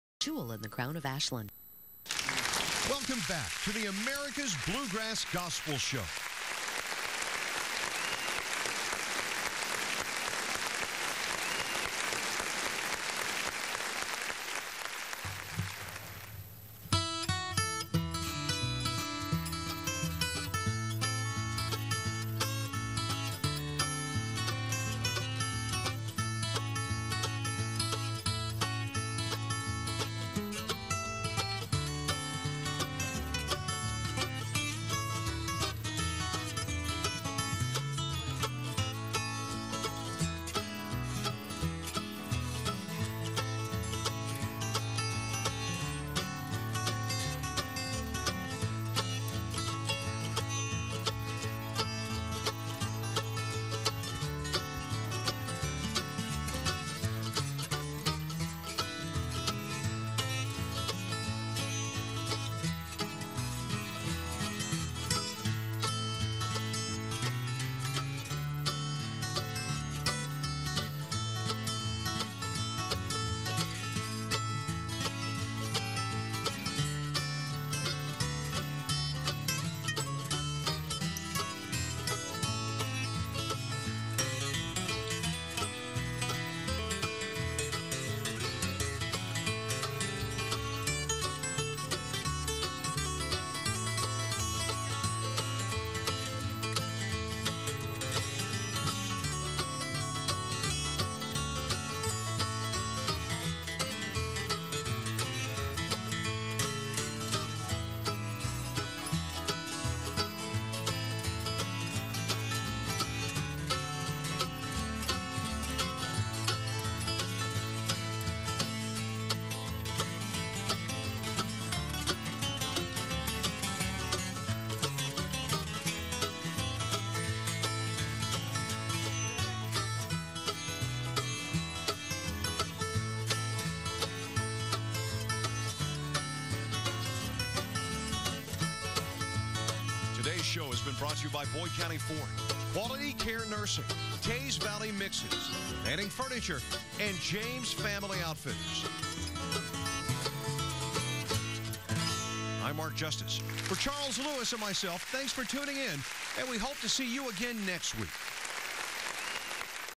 Jack Hicks and The Bluegrass Gospel show.wmv
Jack doing some three finger picking with the America's Bluegrass Band